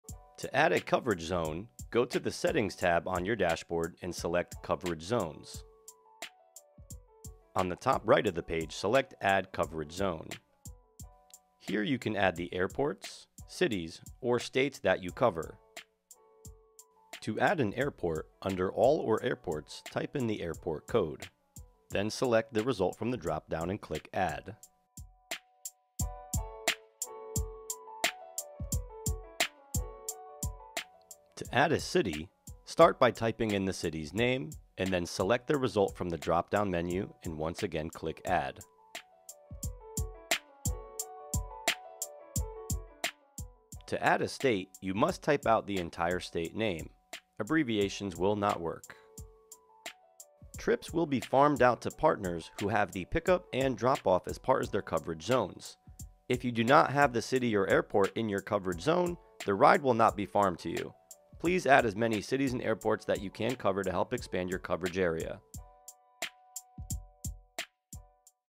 Adding Coverage Zones | LUXY Driver
In this video, you will see how to add coverage zones to your partner dashboard. The more cities and airports you input, the more opportunity you will have to claim rides.